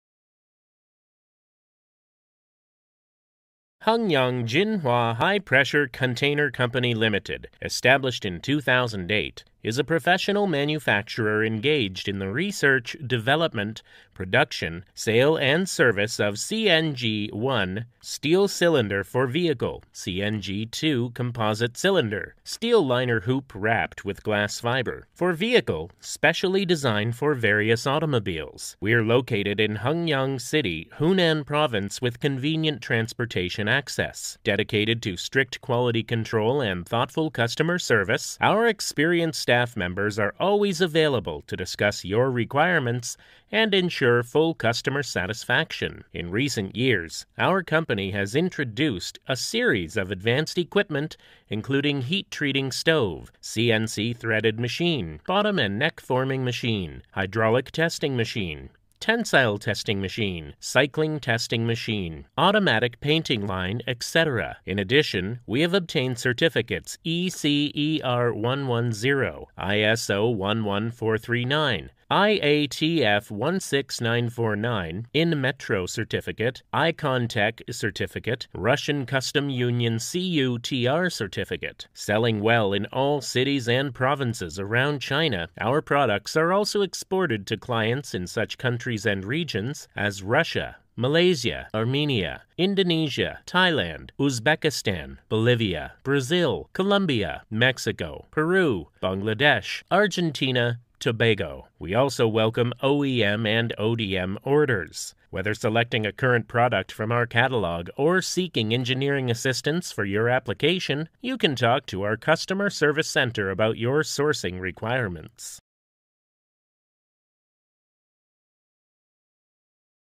Vertified by SGS:Hengyang JinHua High Pressure Container Co., Ltd
Hengyang JinHua High Pressure Container Co., Ltd, established on 2008 and committed to the research and development, manufacturing and sales of steel high pressure cylinder, and is one of the major manufacturers of wide range of CNG Cylinders, specially design for various automobiles.